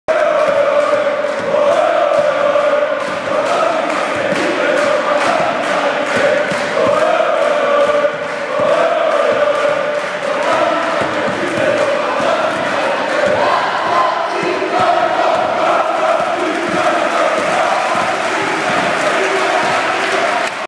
Ημέρα Μνήμης για το Στρατηγό: Π.Α.Ο-Α.Ο Αμύντας
Στο τέλος του πρώτου ημιχρόνου η γυναικεία ομάδα μπάσκετ έκανε το πρώτο ζντο με τον κόσμο!